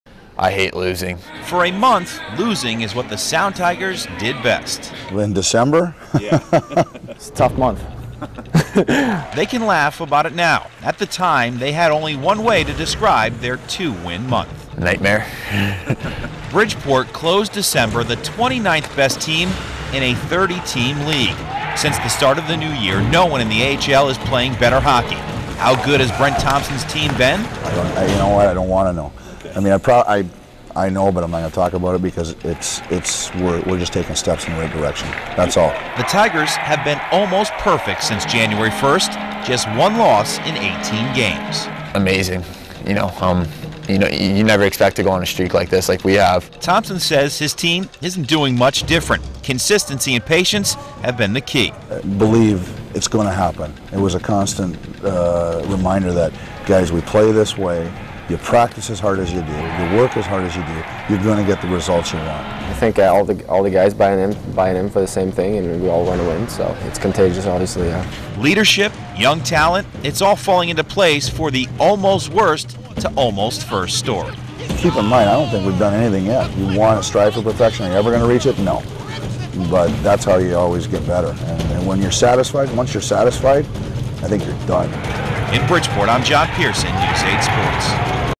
Sound Tigers hit a hot streak
If at first you don't succeed, try try again: Bridgeport's pro hockey team, the Sound Tigers, didn't succeed at first, but they are now.